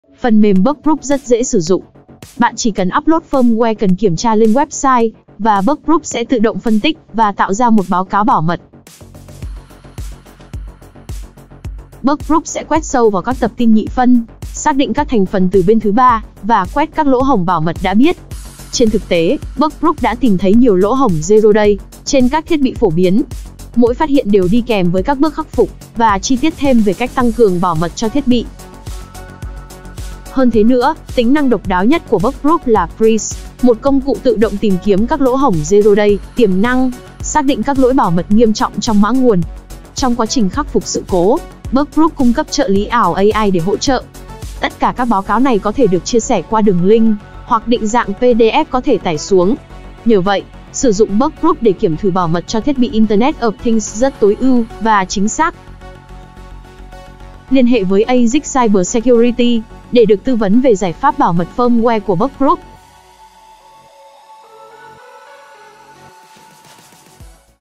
Bugprove - Nền tảng tự động hóa kiểm thử bảo mật firmware
Ngày nay, khi các thiết bị IoT trở nên phổ biến, làm thế nào để các nhà cung cấp chắc chắn rằng các thiết bị IoT đảm bảo an toàn trước các lỗ hổng bảo mật? Bugprove chính là giải pháp giúp kiểm thử và bảo mật firmware một cách dễ dàng và hiệu quả! Tính năng nổi bật:
✨ Quét để tìm các lỗ hổng: Tìm lỗ hổng zero-day trong firmware trước khi bị hacker phát hiện và khai thác. Tìm ra các thành phần phụ thuộc dễ bị tấn công, lỗi lập trình, cấu hình sai và các vấn đề bảo mật phổ biến khác.
✨ Báo cáo và khắc phục: Công cụ dựa trên trí tuệ nhân tạo để hiểu và sửa các lỗ hổng. Báo cáo chi tiết, thông tin đầy đủ cho người dùng.
✨ Giám sát hệ thống 24/7: BugProve theo dõi các lỗ hổng mới phát hiện trong các dòng sản phẩm và cung cấp các bản cập nhật trong thời gian sớm nhất.
📭 Hanoi Office: 17th floor - G3, Vinhomes Green Bay, Me Tri, Hanoi
📭 HCM Office: 3th floor, SCETPA Building, 19A Cong Hoa, HCM